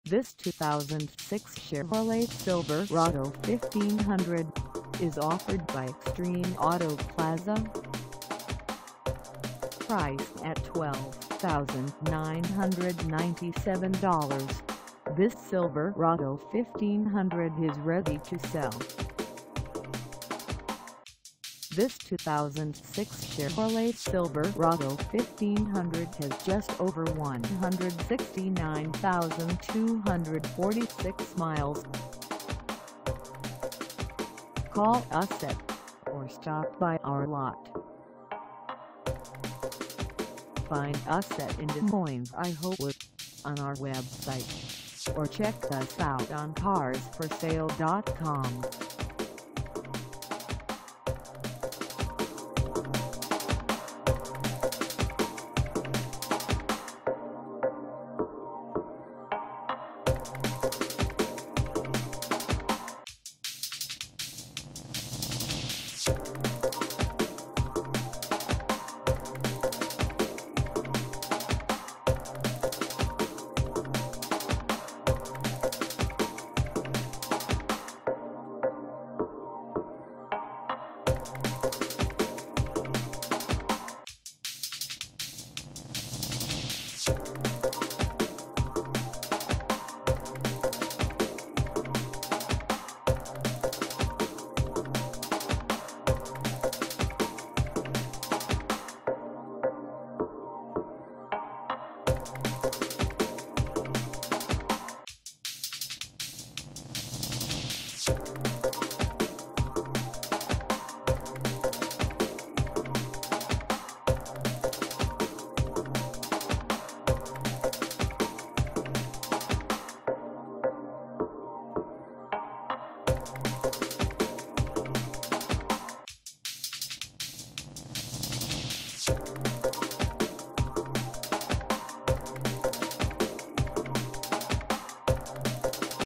2006 Chevrolet Silverado 1500 for sale in Des Moines, IA 503
This 2006 Chevrolet Silverado 1500 is for sale in Des Moines, IA 50316 at Extreme Auto Plaza.
Engine: V8 5.3L V8
Transmission: Automatic 4-Speed
Color: BLACK
Mileage: 169246
Contact Extreme Auto Plaza  Des Moines, IA 50316 to test drive this 2006 Chevrolet Silverado 1500  today.